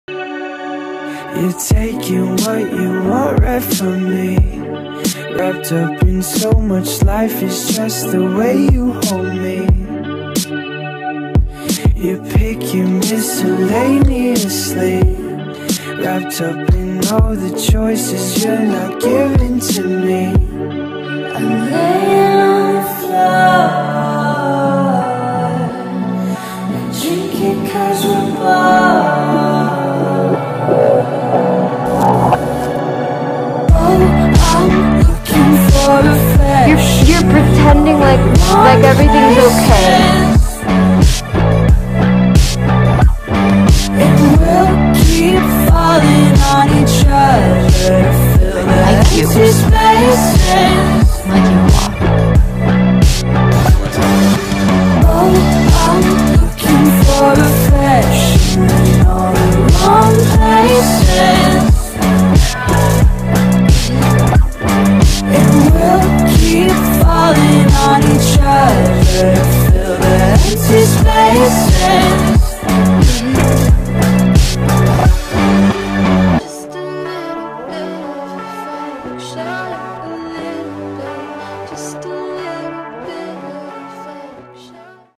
just a little bit of affection
a lil vent video because i'm feeling lonely and dissociative...